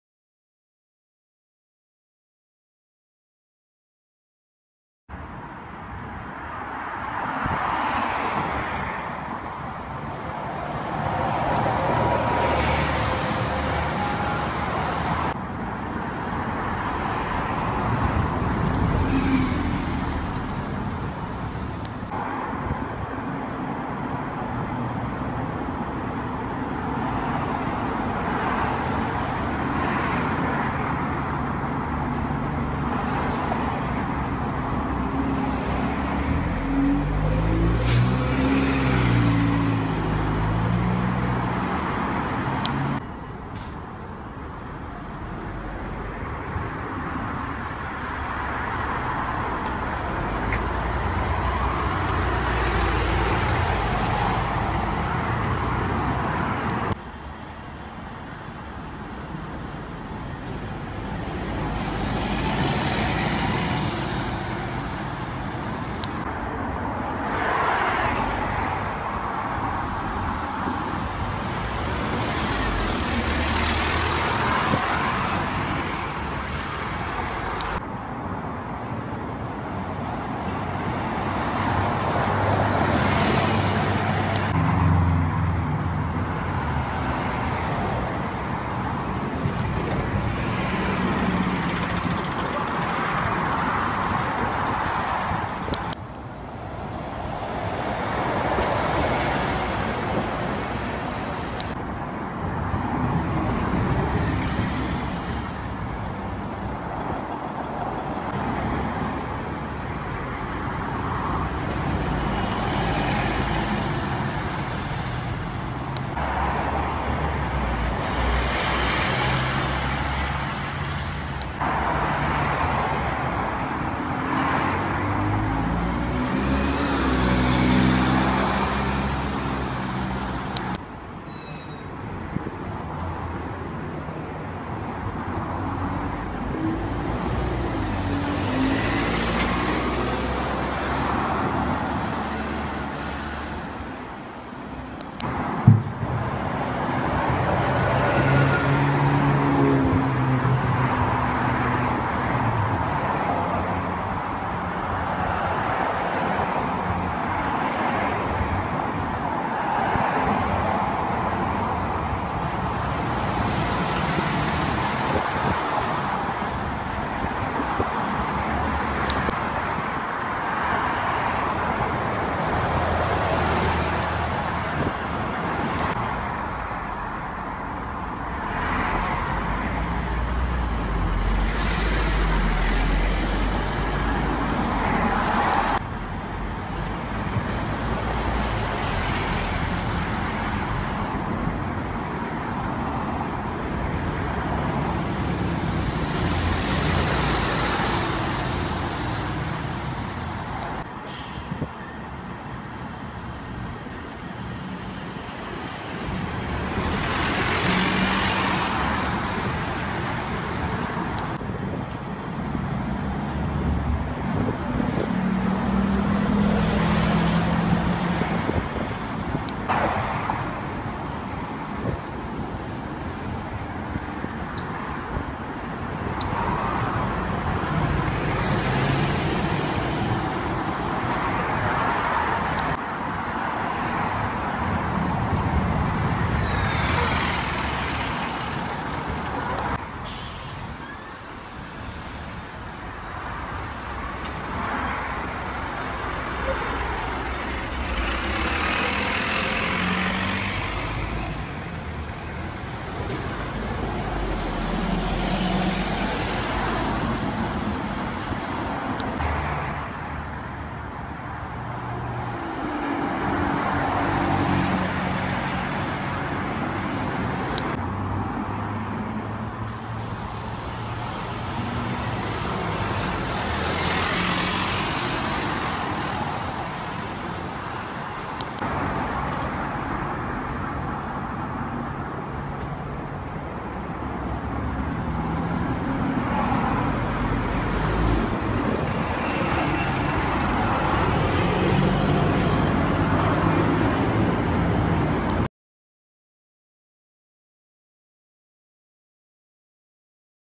Buses at Gatwick South Terminal (A23) (06/07/2019)
Here we have a few buses filmed at Gatwick airport on a fine Saturday afternoon. I will also film at the North Terminal soon as there will be more buses and coaches there.